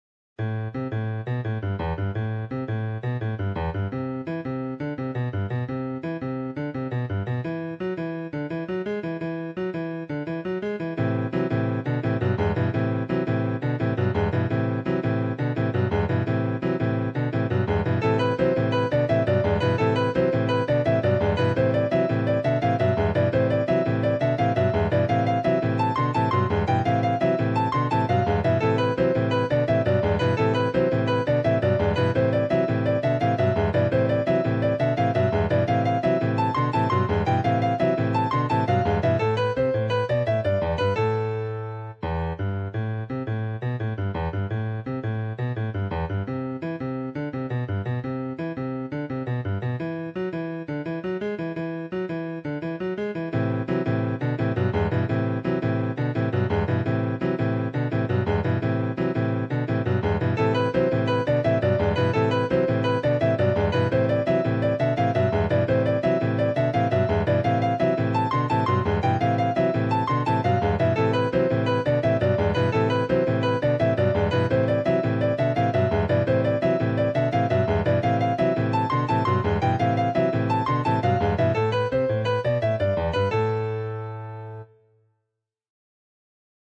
Jazzon | Brighton Music Studios
This song is in 5/4 time. That just means that there are 5 quarter notes in one bar. 3/4 and 4/4 are the most common time signatures, while 2/4 (Cut time) is also used sometimes. There are many other time signatures, but 4/4 is the most common one. I made this song with noteflight, so I will share the pdf for the song in the comment section. If you are thinking about playing this song, I must tell you this: One person cannot play this song, you must have two people to play it, one playing the bass and the other playing treble. This is because I have one chord that contains a minor ninth, which cannot be easily (or at all) played with one hand. This means that you need two hands to play the bass and one hand to play treble, and most people have just two hands, so two pianists are needed. Anyway, enjoy!